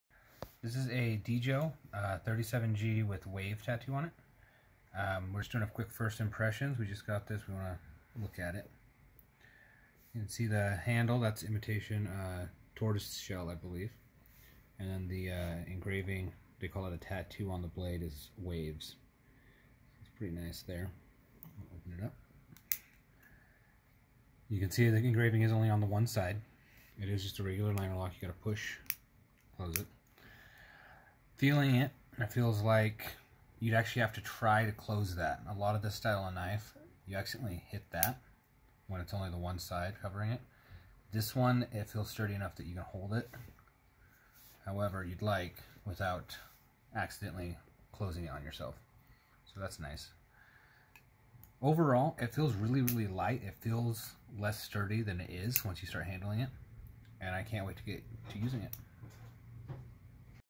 First Impression - Deejo Tattoo Linerlock 37G Wave
A quick video of me talking about my first impressions of the Tattoo Linerlock 37g wave by Deejo.